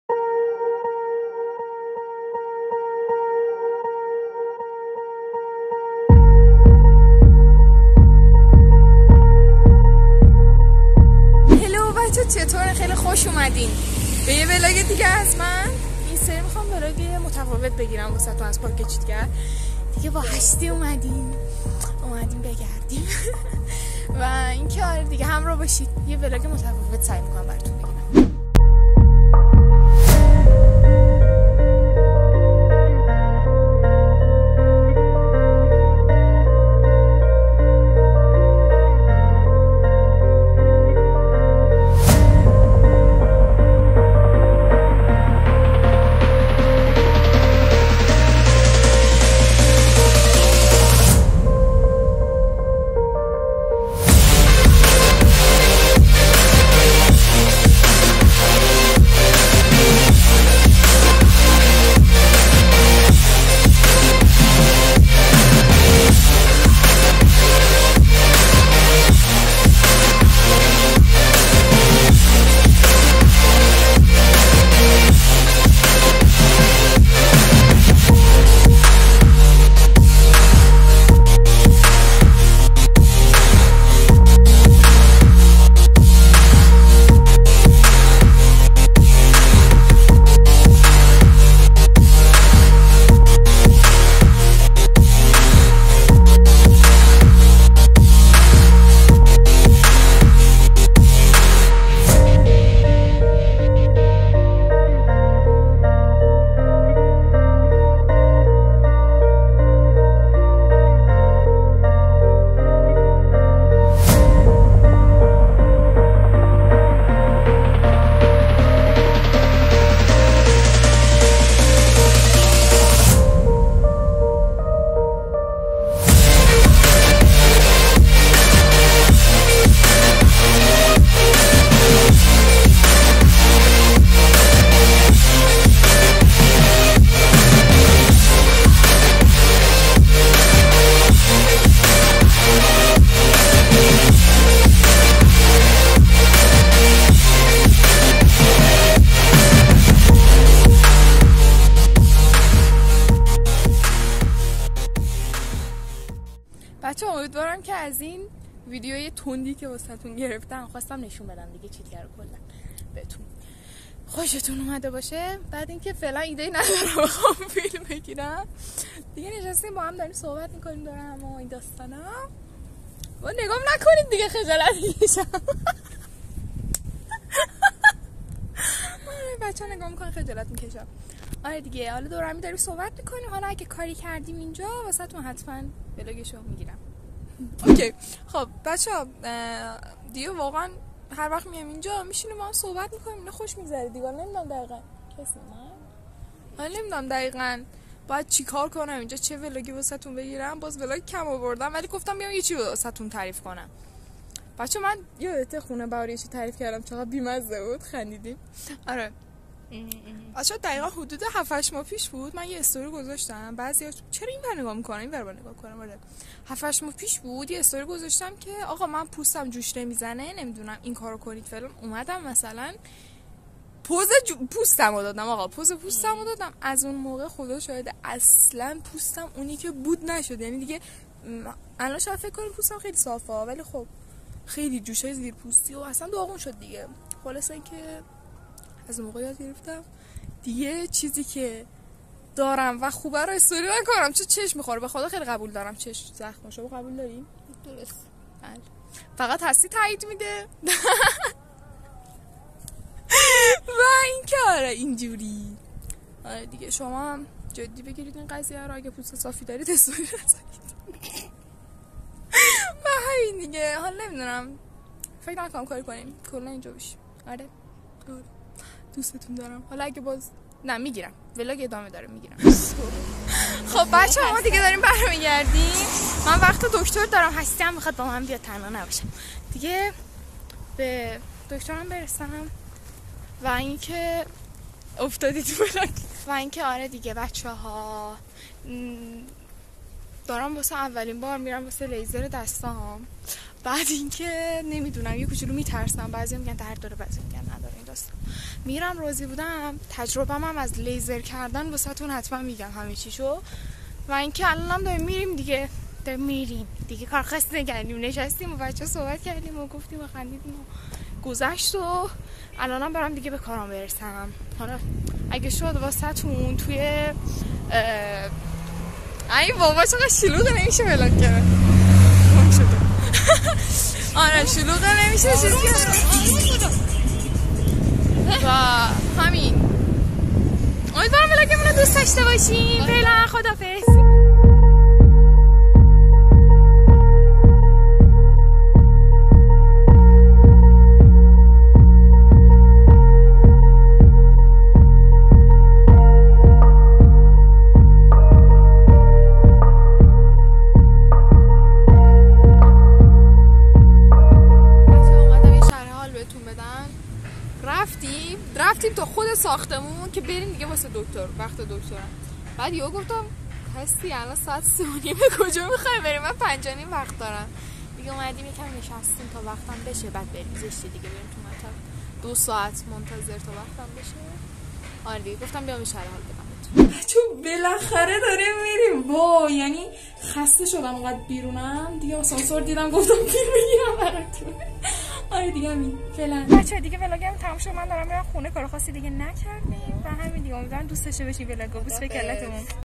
vlog|ولاگ پارک جنگلی چیتگر 2
امیدوارم ویدیورو دوست داشته باشین
ایده ای اگه دارین برام بنویسین این سری رفتم چیتگر براتون ویدیو اماده کنم🤌🏻🥲